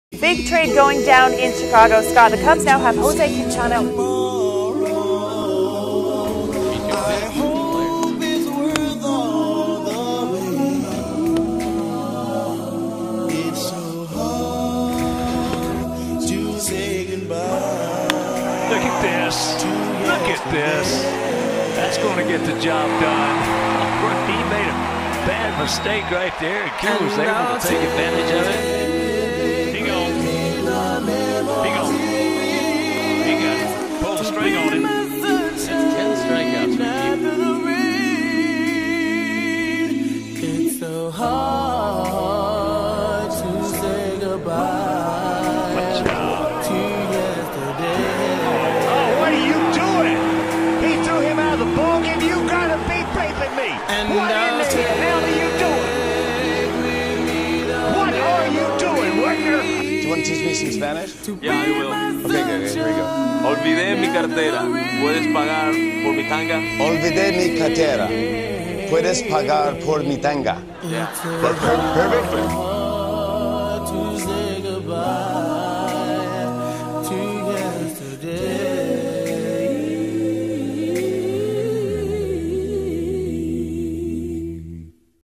Jose Quintana-Thanks for The Memories
Jose started his White Sox career in 2012. In 2016, he was named to the AL All Star team. Jose, we will truly miss you and wish you the best of luck on the Cubs, except in the crosstown cup. Thanks for the great memories, Jose.